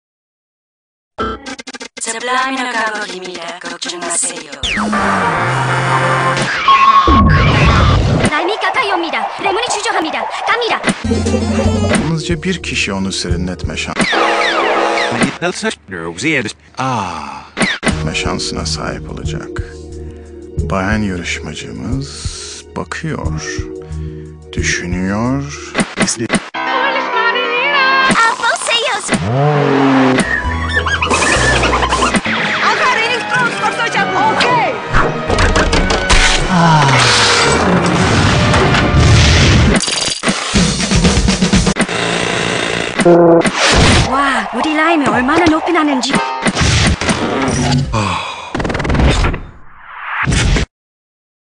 Sprite Sublymonal - Foreign Tounges (RARE)
a rare ad from Sprite, From the Sublymonal Campaign